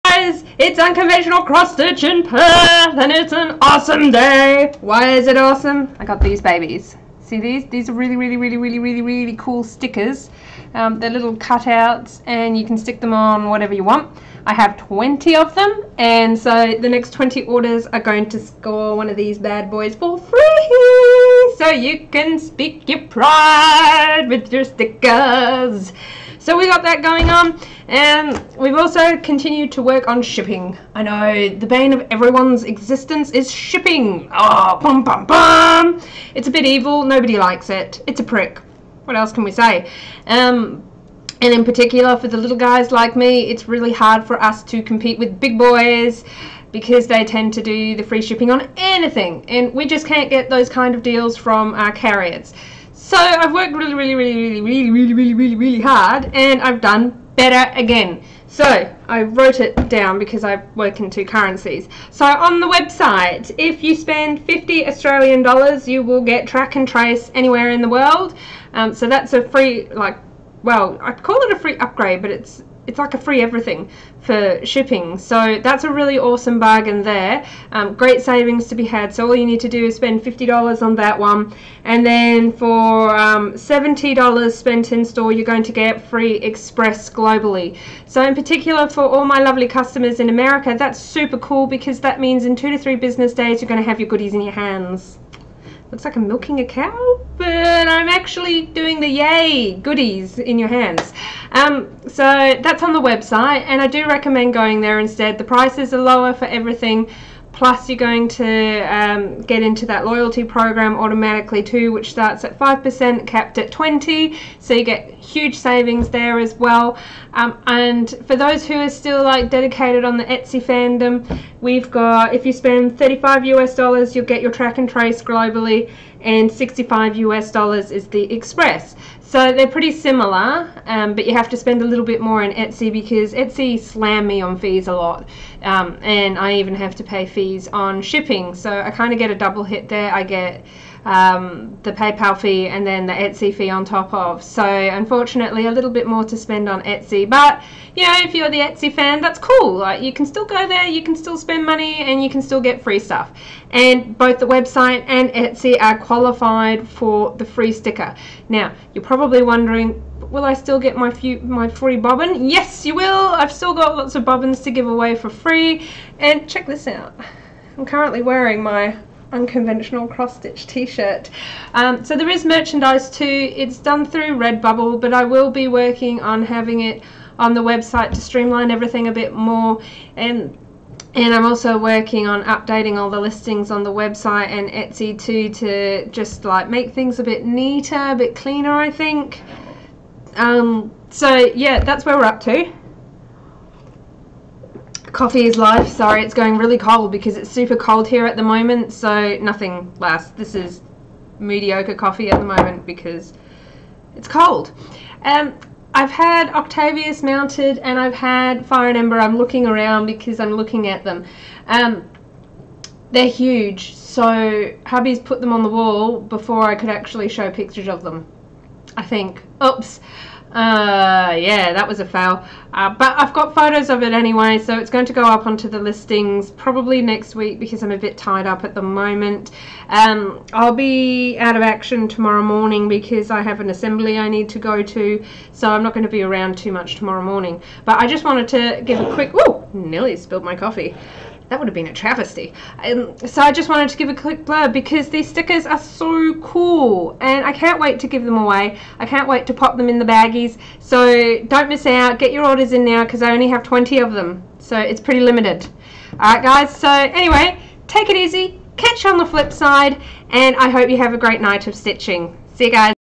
FREEEEEE stuffs from Unconventional X Stitch
Who doesn't love free? Get your shop on and don't miss out on these wicked stickers!